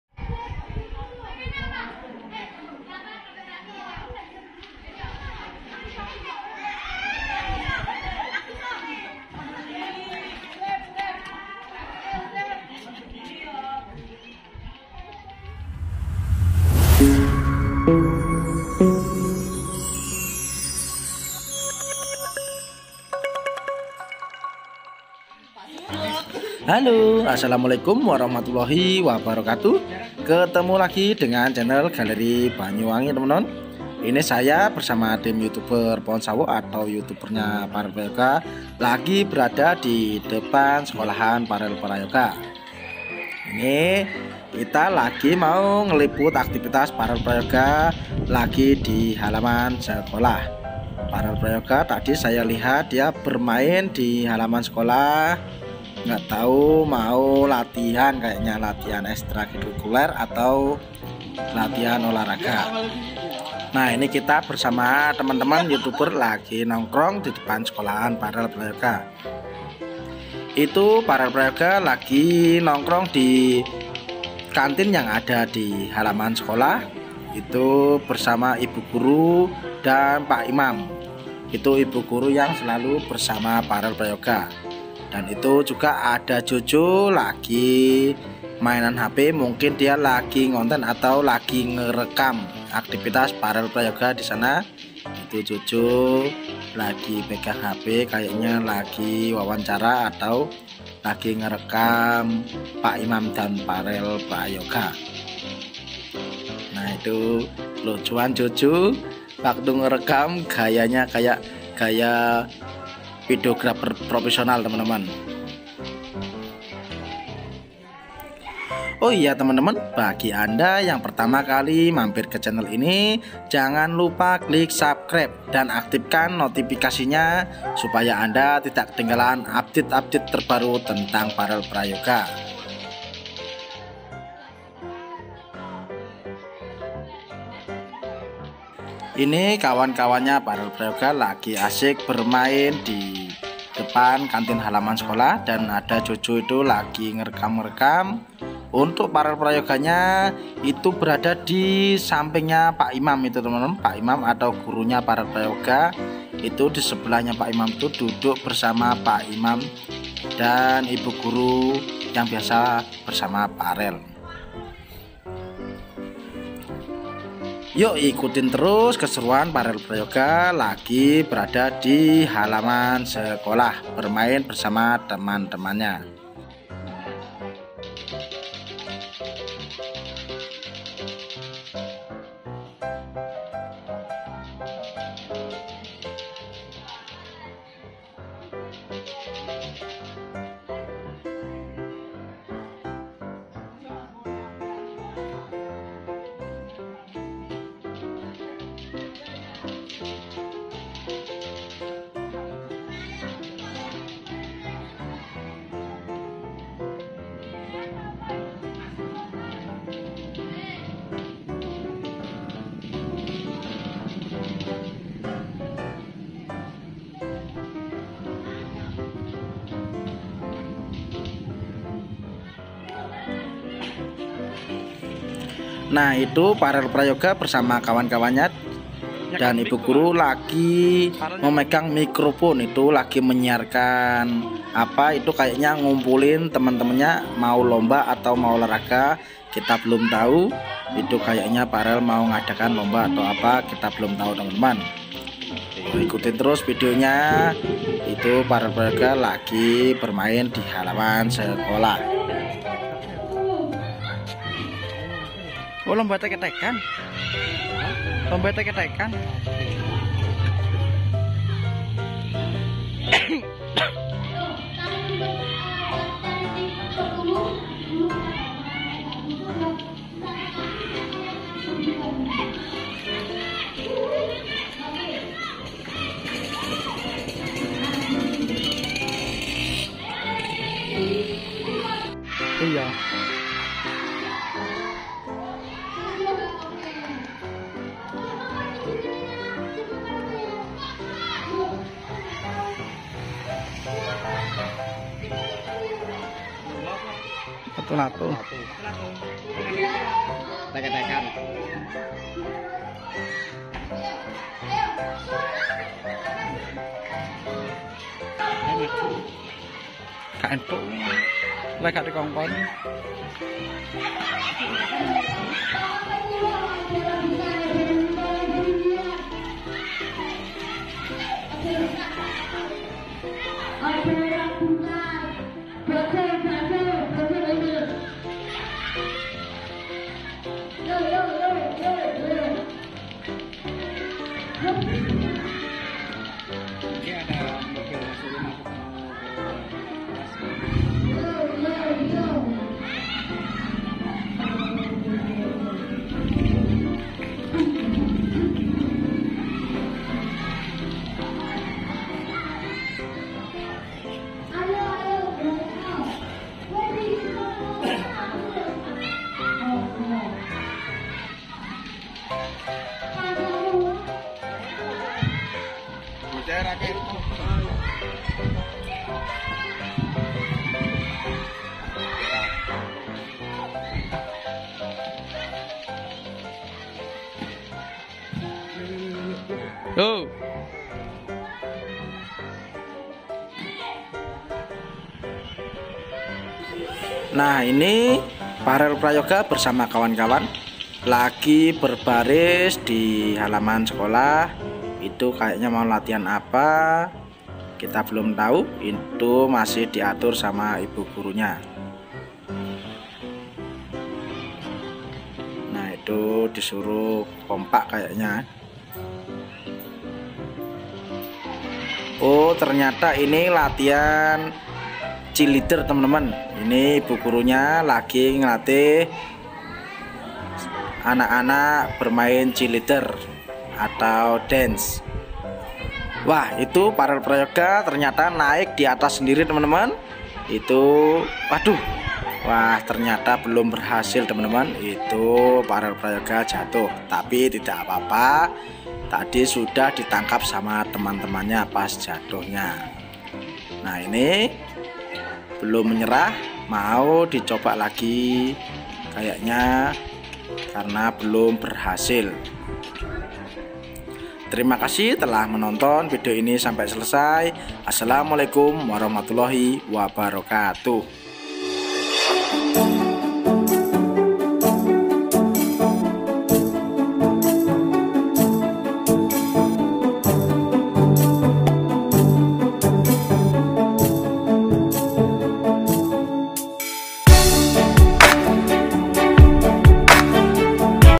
DETIK DETIK‼️FAREL PRAYOGA TERJATUH SAAT LATIHAN CHEERLEADER
detik-detik Farel Prayoga terjatuh saat latihan cheerleader tapi Farel Prayoga tetap semangat latihan bersama kawan-kawan dan dipandu oleh ibu guru sekolah